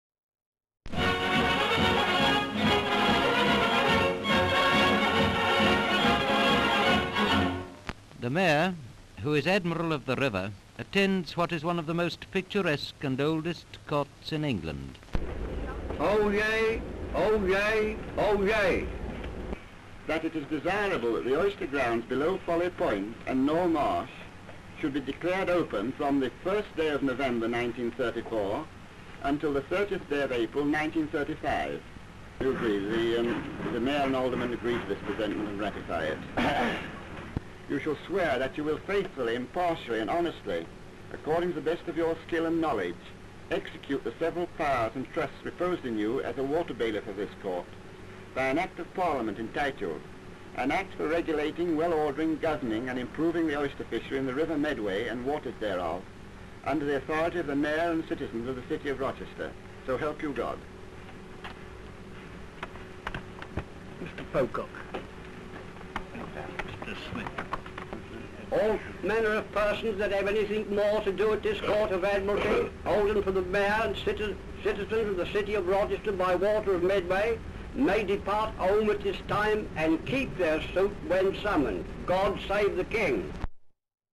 Court On A Barge (1934)
Full title reads: "COURT ON A BARGE - Medway Court of Administration controlling 20 miles of river fisheries sits at Rochester."

Rochester, Kent.

Mayor and several court officials arriving to a barge court at Rochester. Various shots of the open air barge court in session.
 FILM ID:1658.04